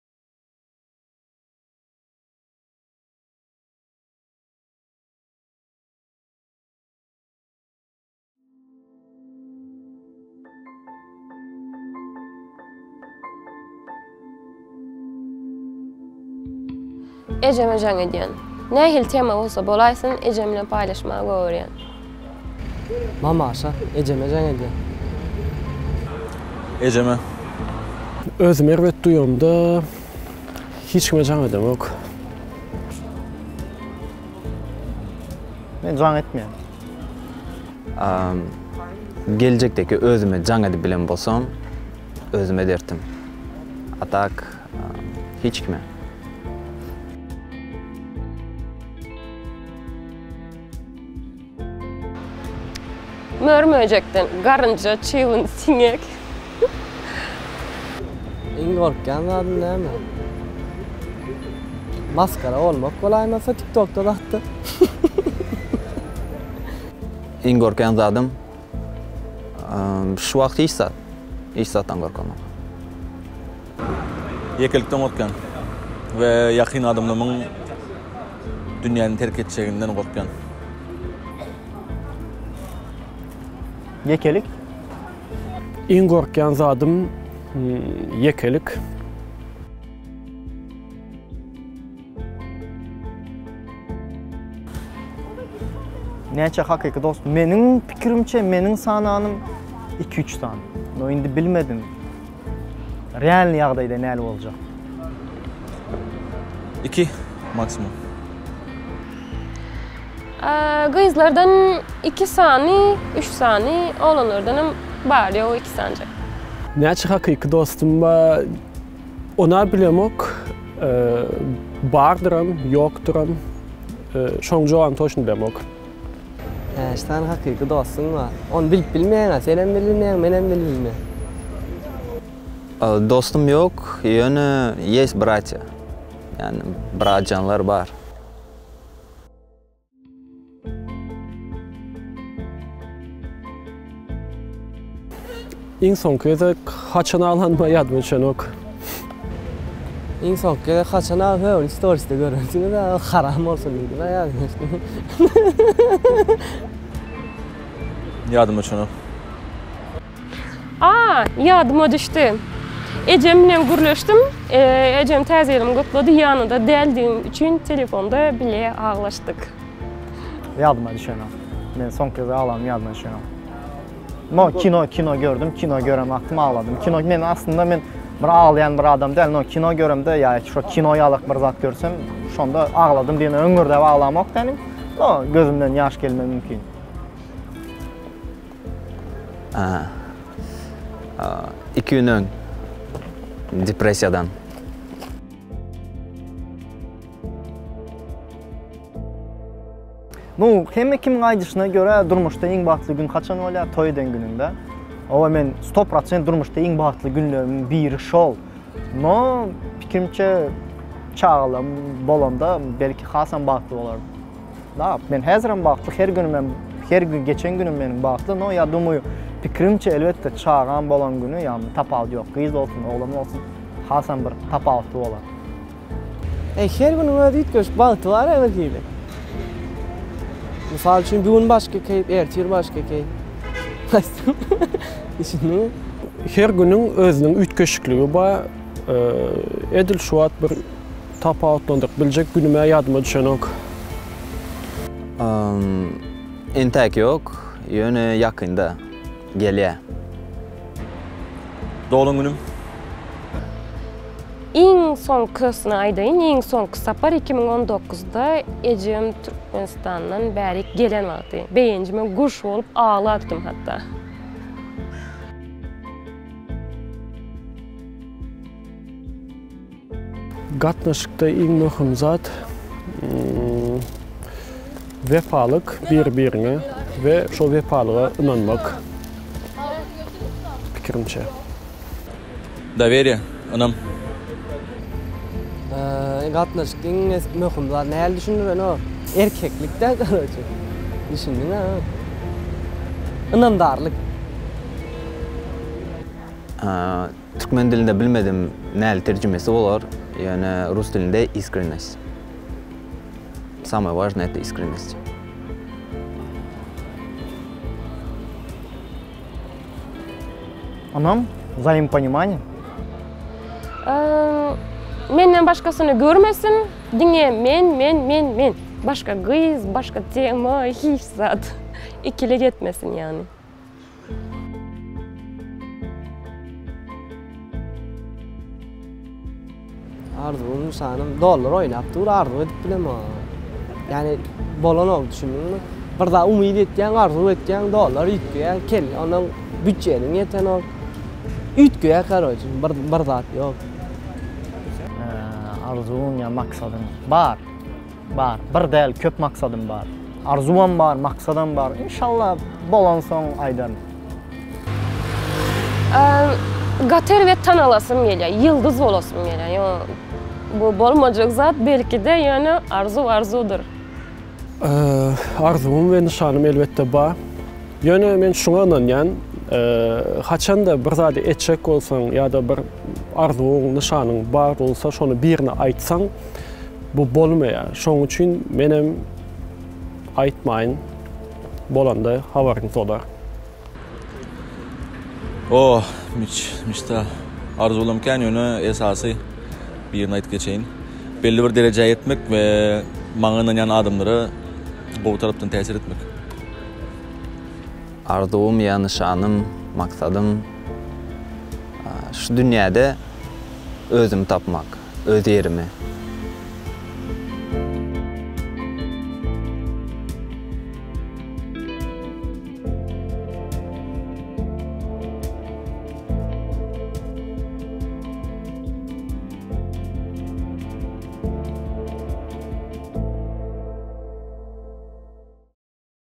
Nurana Arslanowa | Men Dowran | JT Nur | Gaipov | Swizzy347 | Betbelli | 7Sorag
Nurana Arslanova / Men Dovran / Jt Nur / Gaipow / Swizzy347 / Betbelli /

Interwýu formatda täze gepleşik - 7Sorag.